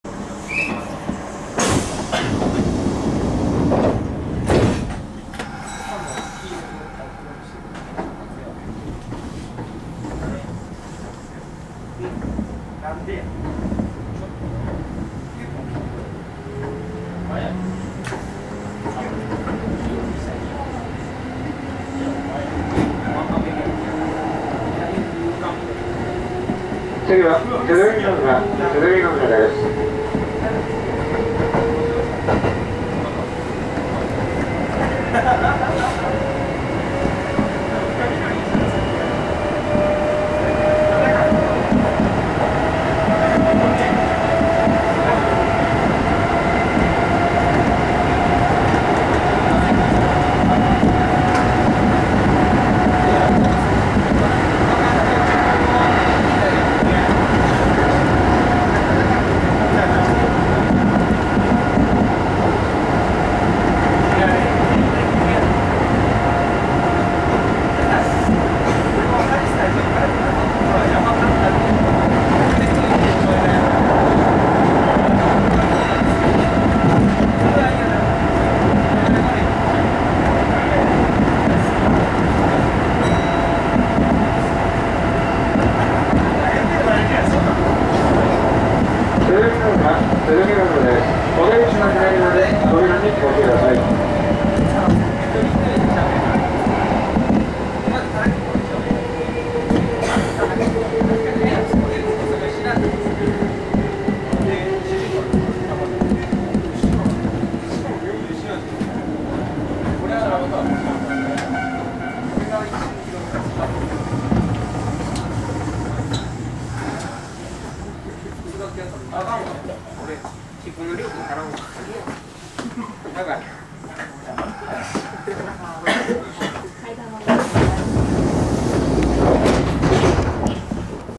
近鉄名古屋線普通磯山⇒鼓ヶ浦
撮影地：磯山⇒鼓ヶ浦
車両：近鉄1000系